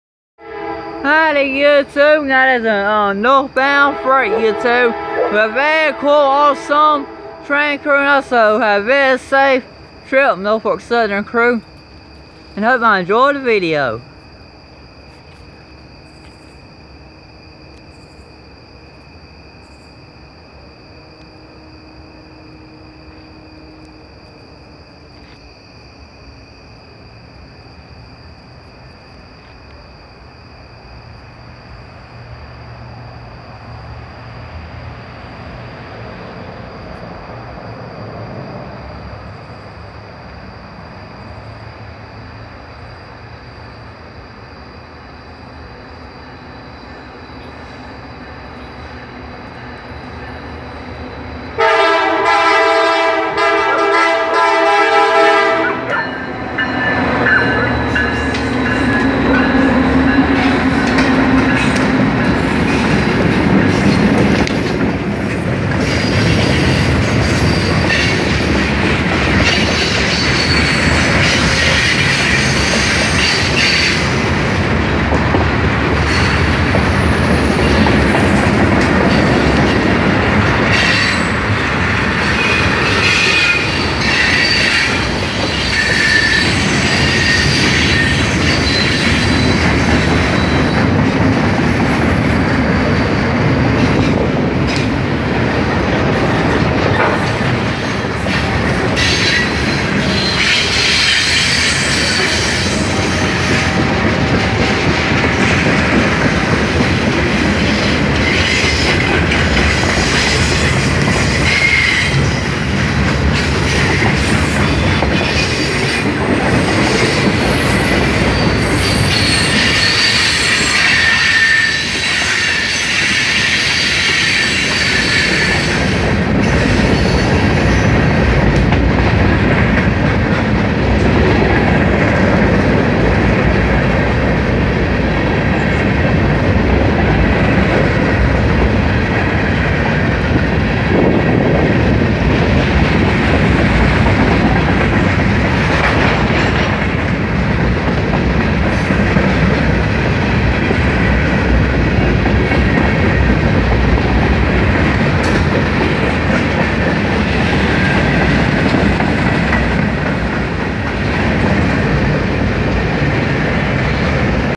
Norfolk Southern led NS mixed manifest freight train in Griffin Ga
Have a Safe Trip Norfolk Southern crew.